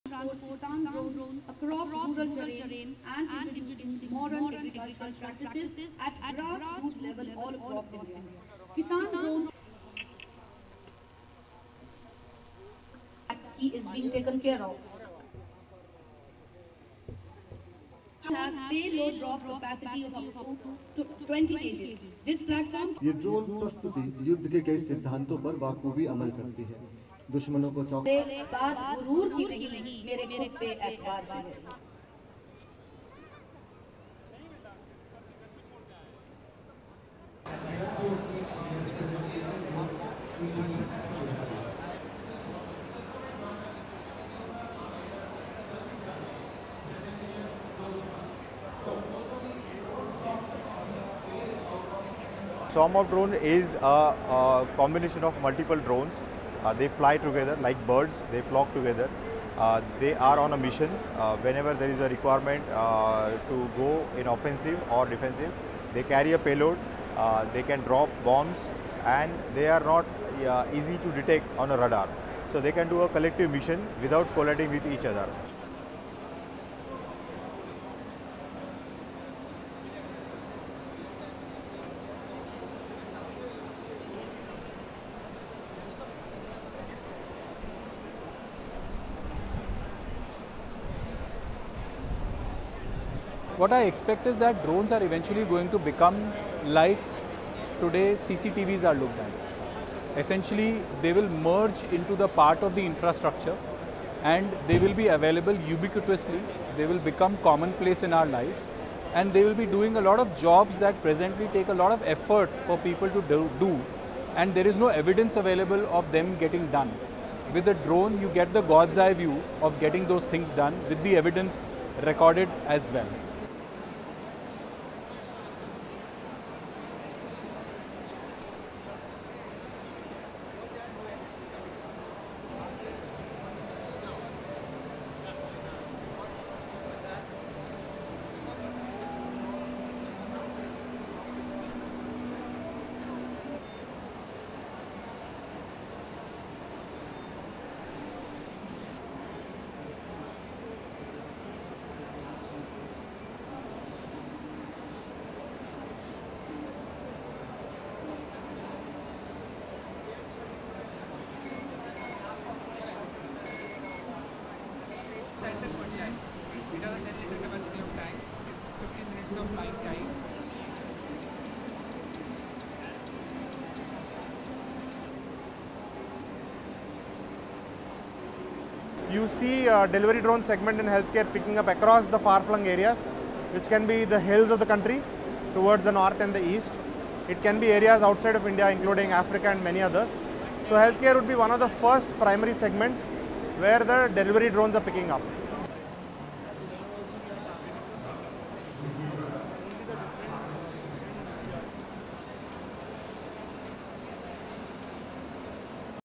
India drone show showcases homegrown technology
INDIA DRONE SHOW

RESTRICTIONS:

LENGTH: 3:26

ASSOCIATED PRESS
Hindan Airbase, Uttar Pradesh - 25 September 2023
1. Demonstration of concept drone used by farmers to spray pesticides on fields
2. Mid of a person sitting on ground
3. Mid of a demonstration of concept drone used to transport food, medicines and other logistics items with payload capacity of 20kg
4. Various of demonstration of swarm drones - a number of drones used in a pattern with potential in modern warfare
5. Wide of exterior of the exhibition hall
6. Mid of people at exhibition
7. Various of swarm drones on display by Ayaan Autonomous Systems
8. SOUNDBITE (English) Atul Chaudhary, CEO, Ayaan Autonomous Systems:
“Swarm of drones is a combination of multiple drones. They fly together like birds, they flock together. They are on a mission whenever there is a requirement to go in offensive or defensive. They carry a payload, they can drop bombs and they are not easy to detect on a radar. So they can do a collective mission without colliding with each other.”

9. Pan of ‘Switch’, an all terrain drone with thermal vision capabilities
10. Close of poster for Switch drone
11. Various of Netra V4 Pro drone, light weight and multipurpose drone used in mapping and surveying
12. SOUNDBITE (English) Ankit Mehta, Co-founder, Idea Forge:
“What I expect is that drones are eventually going to become like (the way) today CCTVs are looked at. Essentially they will merge into part of the infrastructure and they will be available ubiquitously. They will become commonplace in our lives and they will be doing a lot of jobs that presently take up a lot of effort for people to do. And there is no evidence available of them getting done. With a drone you get the God’s eye view of getting those things done with the evidence recorded as well.“

13. Various of interior of the exhibition hall with drones on display
14. Various of Aero Arc, a multipurpose unmanned aerial vehicle (UAV), used in defense and surveillance
15. Wide of a TV screen showing filmed footage from an Octaglide drone used for transporting goods up to 20kg
16. Wide of police officials talking to drone manufacturers
17. Manufacturer interacting with a visitor
18. Various of drones used for medicine delivery
19. SOUNDBITE (English) Sarath Chandra Gudlavelleti, CEO, Neosky
“You see a delivery drone segment in healthcare picking up across the far flung areas which can be the hills of the country, towards the north and the east. It can be areas outside of India including Africa and many others. So healthcare will be one of the first primary segments where the delivery drones are picking up.”

20. ‘Replacement of Chinese DJI with Indigenous Ajeet Mini Surveillance UAV’ written on the display board of the Indian drone brand ‘Ajeet’
21. Wide of stalls with drone manufacturers and visitors inside the exhibition hall

LEADIN:
Indian tech companies have been showcasing their homegrown drone systems at an exhibition in Uttar Pradesh.

The Unmanned Aerial Vehicles can be used for a range of tasks, from dropping bombs to delivering medicines.

STORYLINE:
There's a buzz in the air at Hindan Airbase in Uttar Pradesh - but that's from flying machines.

The site is hosting Bharat Drone Shakti, a mega drone show.

The Drone Federation of India and The Indian Air Force have organised the event to promote
UAVs (Unmanned Aerial Vehicles) and showcase new drone technologies, both for civilian and defense use.

The Indian drone market reached $1,020 million US in 2022.